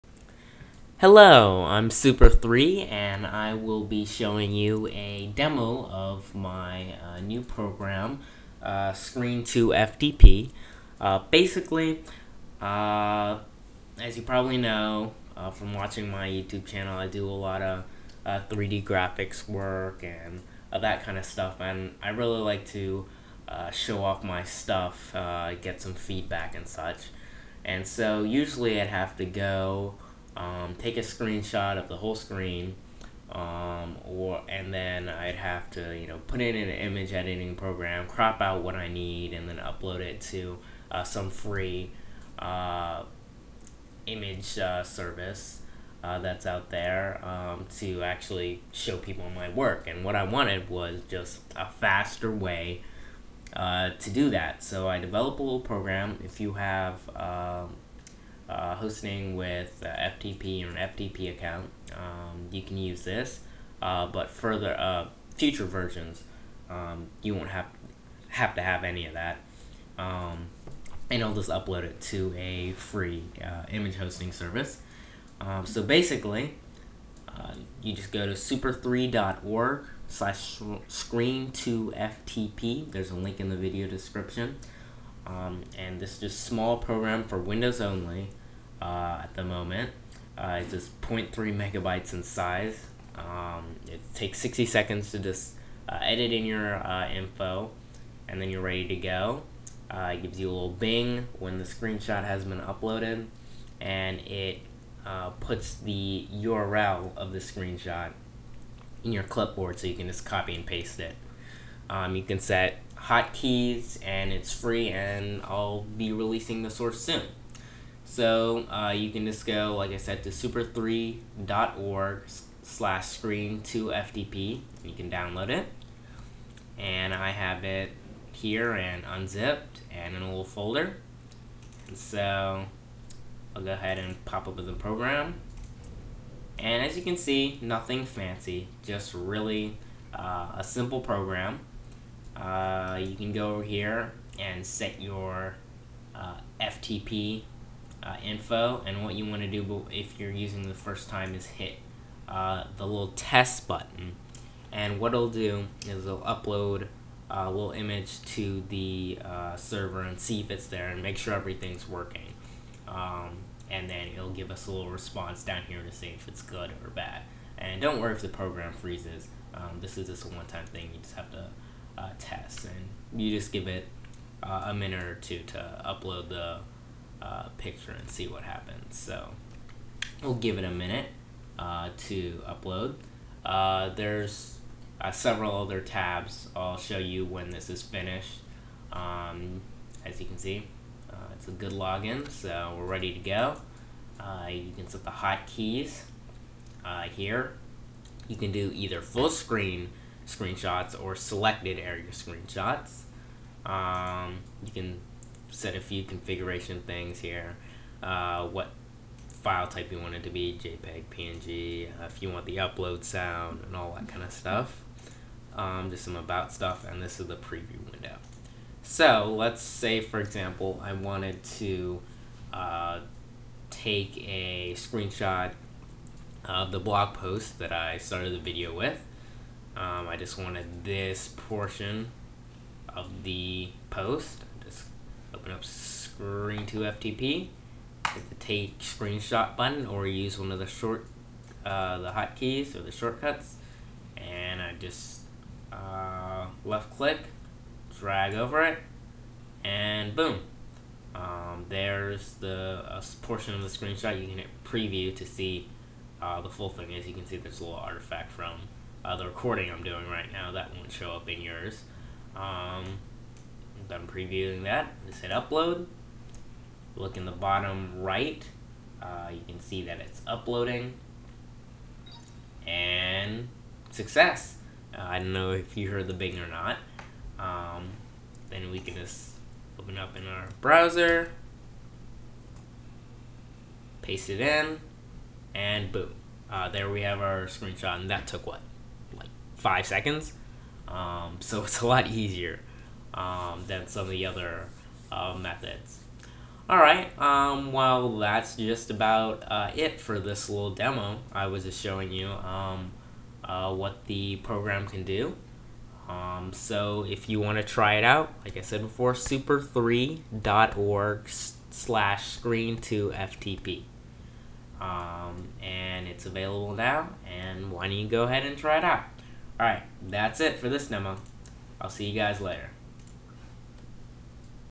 ScreenToFTP Demo
This is part of a software program that I am currently working on as I practice my C#. Hopefully soon it will be a more consumer based product. Right now it is very helpful for talking quick screenshots to share with other people. If you have an FTP server you should probably try it out.